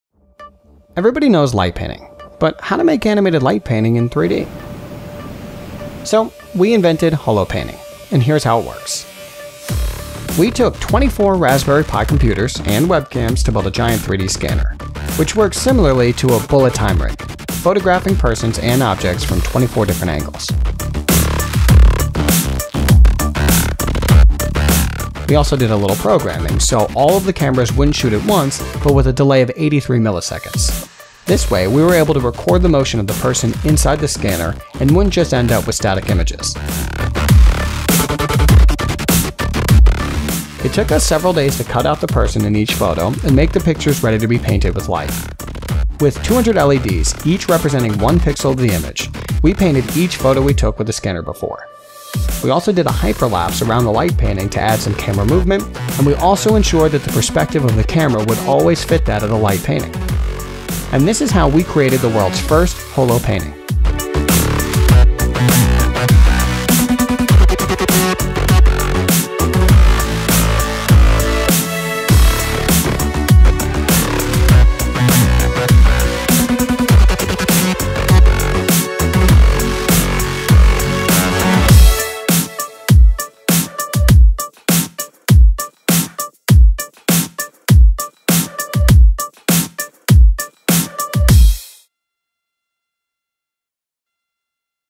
The Invention of HoloPainting
Holopainting is a combination of the Light Painting, Stop Motion and Hyperlapse technique to create three dimensional light paintings.
We didn't want to use computer generated images, so we built a giant 3D scanner out of 24 Raspberry Pis with their webcams. These cameras took photos from 24 different perspectives of the person in the middle with a delay of 83 milliseconds, so the movement of the person also was recorded.
After that we spent endless hours cutting out each photo, so we would have the person on a black solid as background. We then made a hyperlapse around a light painting artist who painted each of the photos we took before according to the current position of the camera. As a result the hyperlapse circulates around a three dimensional light painting. We felt like painting a hologram, so we called it a holopainting.

Check out our smartphone app to turn an Android smartphone into a longterm timelapse camera

This is our graduation project for our university.
Thanks to the University of Applied Science St. Pölten for supporting this project.
Thanks to our client HOERBIGER for letting us use one of their old facilities to do the holopainting.
Thanks to the FFG which sponsored the Raspberry Pis, Webcams and wires to build the 3D-Scanner.

Equipment used:
3D Scanner:
+ 24 Raspberry Pis
+ 24 Raspberry Pi webcams
+ 1 48-Port-Switch
+ 375 meters of LAN cable

Holopainting:
+ Canon 6D
+ Sachtler Ace M
+ Pixelstick

Making Of:
+ Canon 5D MkIII
+ Sony a7rII
+ Sony a6300
+ Came-TV Single